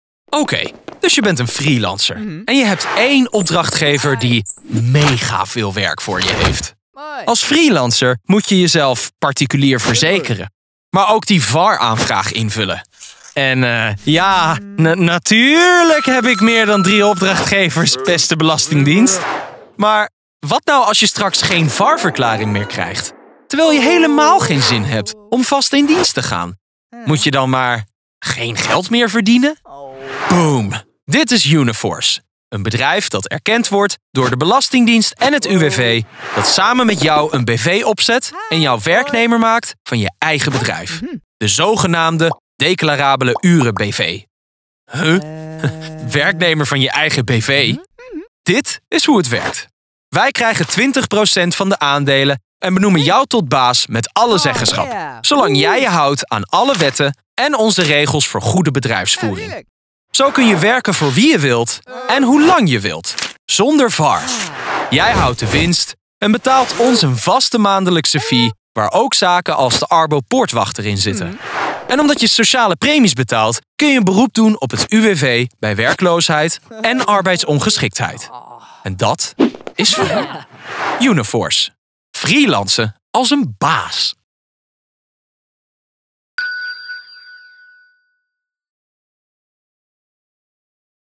Uniforce helpt freelancers te kunnen werken door een eigen BV voor ze te maken
Uniforce is een bedrijf dat erkend wordt door de belanstingdienst en het UWV, dat samen met jou een BV opzet en jou werknemer maakt van je eigen bedrijf.

Produced by Funk-e animations